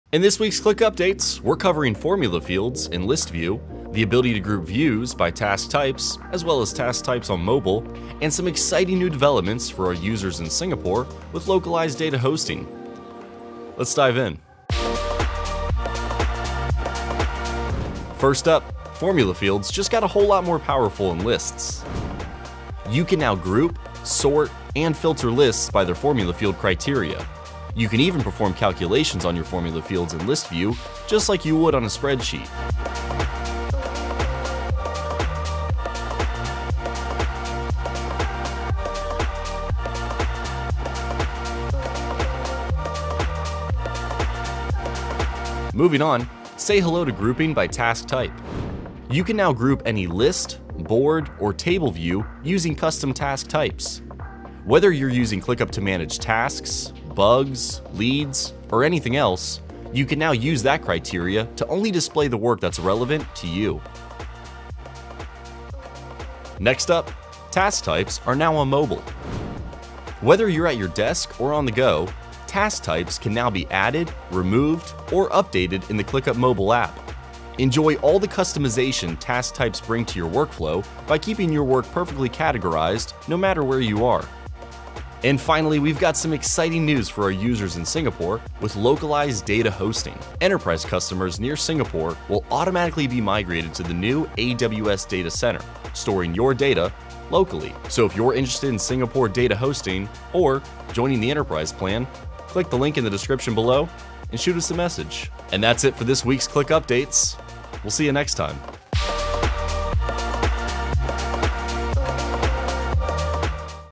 ClickUp Release 3.10
There's even more power packed into your Formula Fields. 💥

Our latest feature release includes calculations for Formula Field columns in your Lists, plus sort and filter options! Task Types also got a major update: Now you get even more flexibility to group any List view, Board view, or Table view with custom task types. 😎

Also new:

🟣 Update task types on mobile
🟣 Localized data hosting in Singapore 🇸🇬
🟣 Bug fixes, templates & on-demand webinars